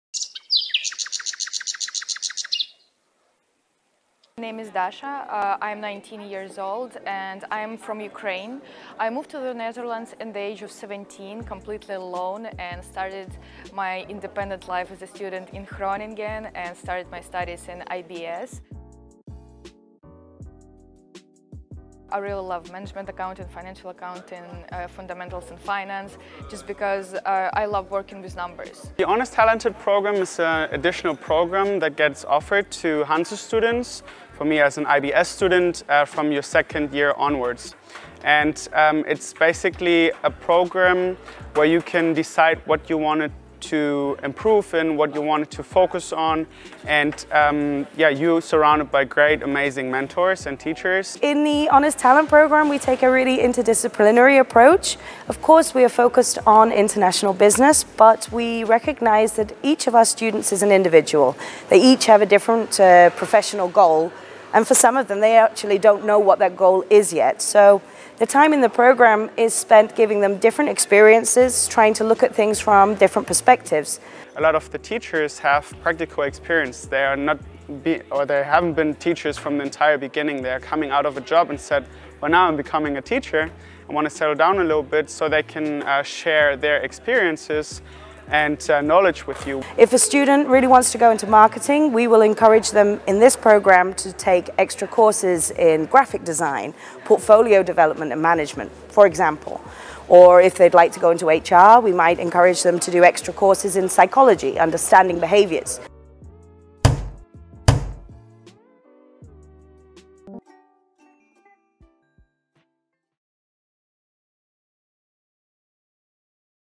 International Business year 2 and Honours programme, Hanze UAS Groningen
Second year IB students and lecturers talk about their experiences at IBS and in the Honours Talent Programme. One of the lecturers and coordinator of the IBS Honours Programme explains what this programme is about.